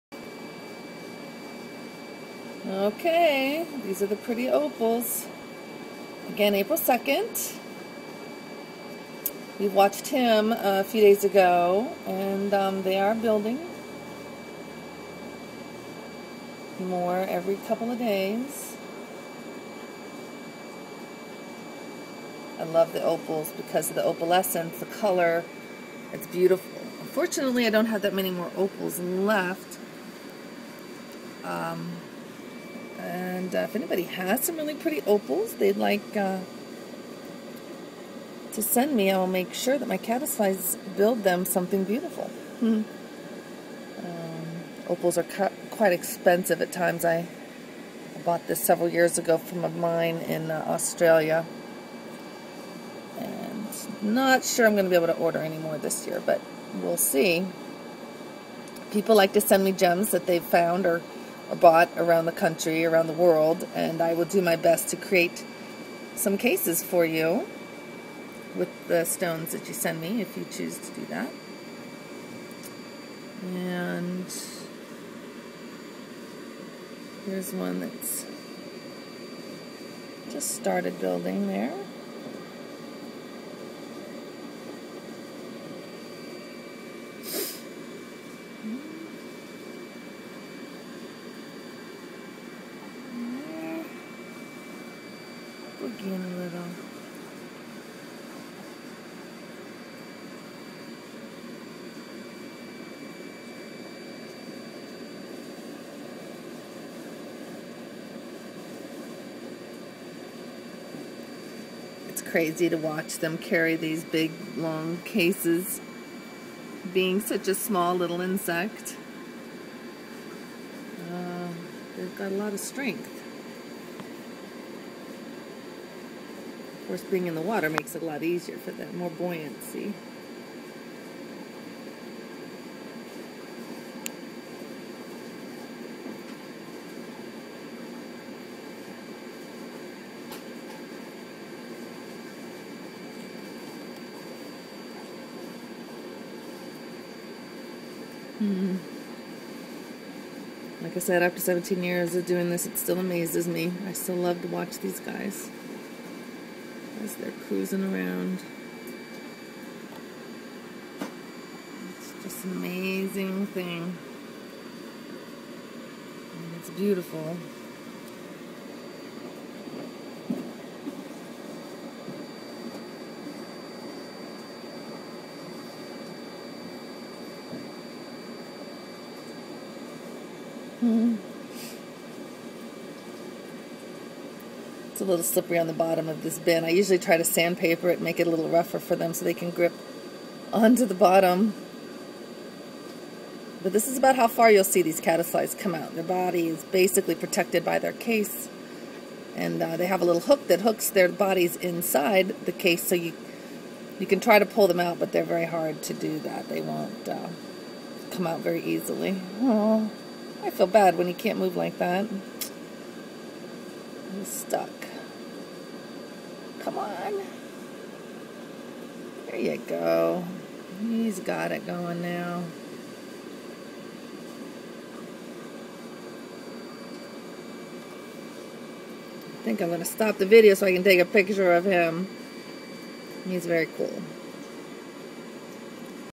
Caddisfly larva building with opals
opal cases with the caddisfly larva.  just beautiful!